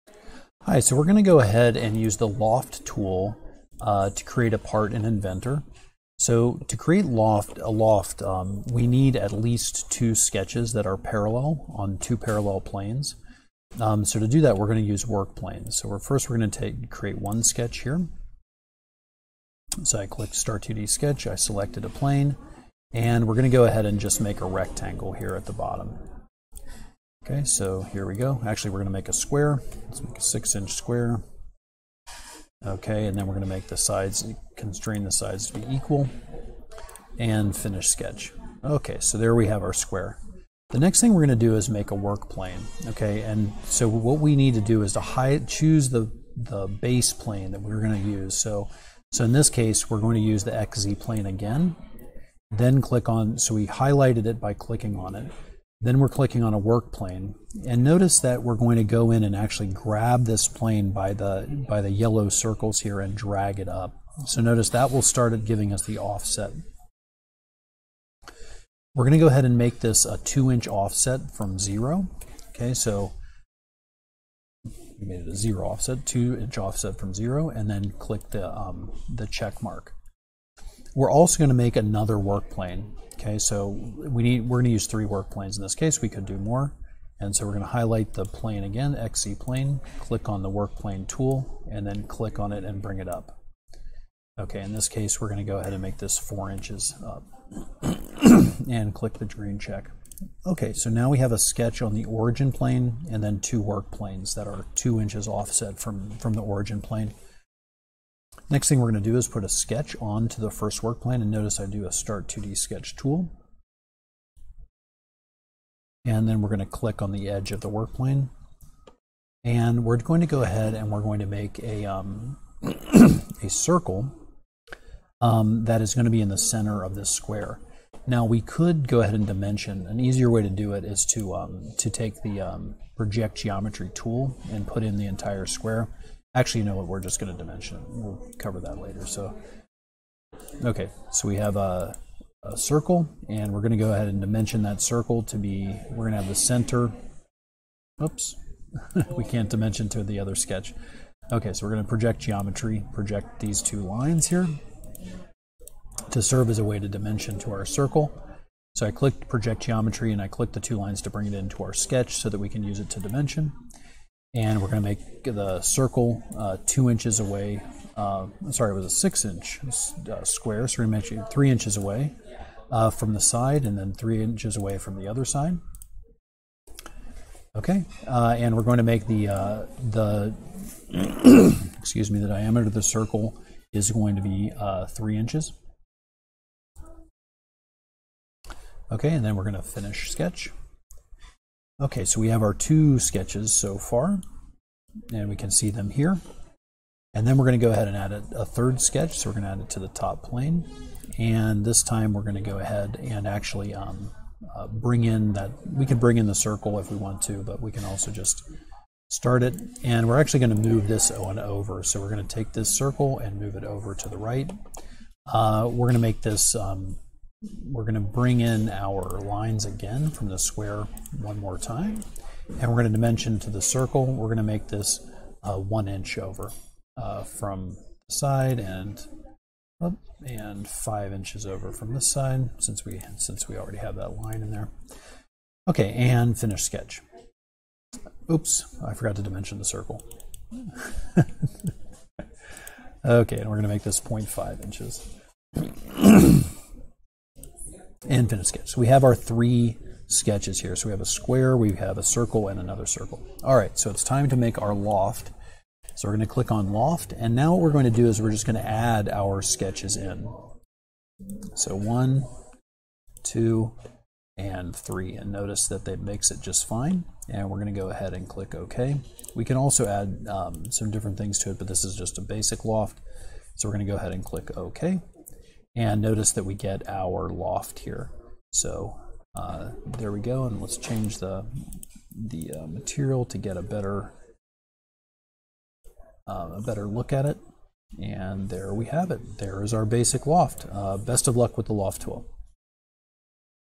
Autodesk Inventor 2020: 9: 3D Loft
This video is part of a basic course in Autodesk Inventor 2020. This video focuses on the 3D Loft tool .